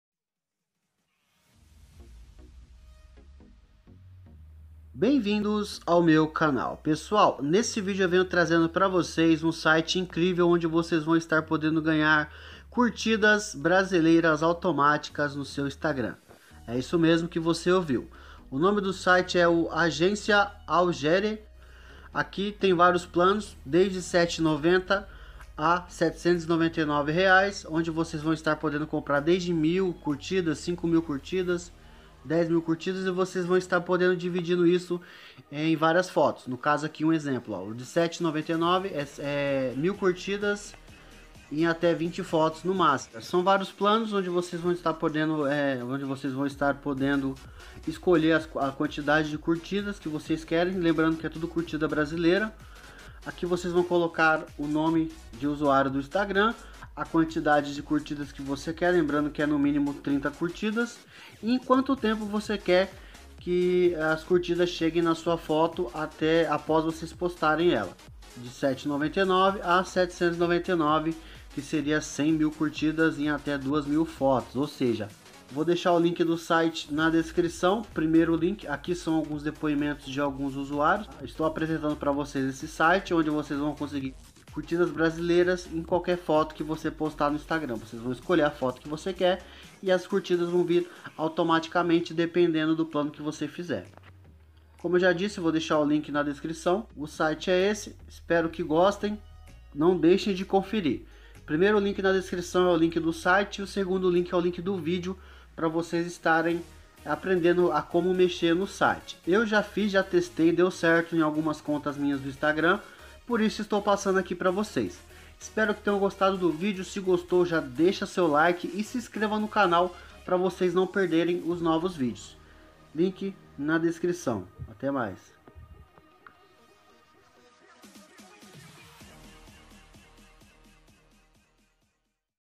Como Aumentar CURTIDAS BRASILEIRAS No INSTAGRAM ATUALIZADO 2018 Melhor Site !!!
para curtidas!

Nesse vídeo eu mostro um ótimo site para conseguir curtidas BRASILEIRAS em fotos no seu Instagram!

Vídeo mostrando como comprar as curtidas !

_____ Ajude o nosso Canal_____

Emagreça de 5 a 8 kg em 21 dias !

CÓDIGO DF45115

_________ Créditos _________

Créditos/ Músicas do canal!